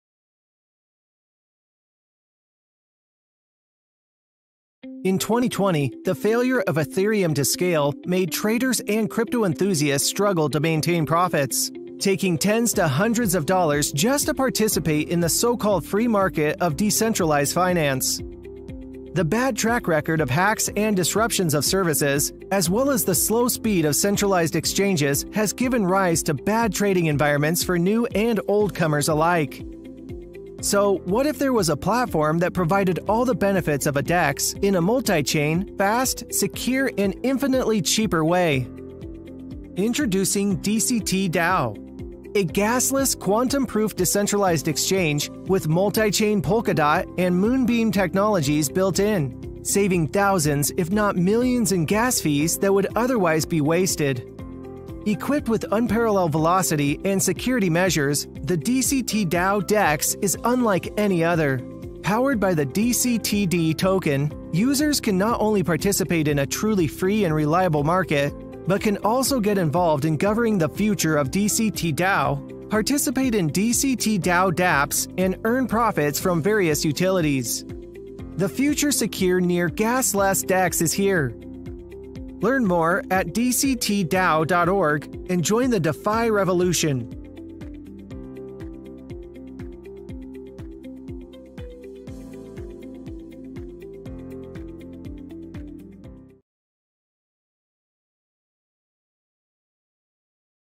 DCTDAO简介
What if we tell you, there is a way to trade quickly, securely, on any crypto you want that will cost you next to nothing?

Then DCTDAO will blow your mind 🤯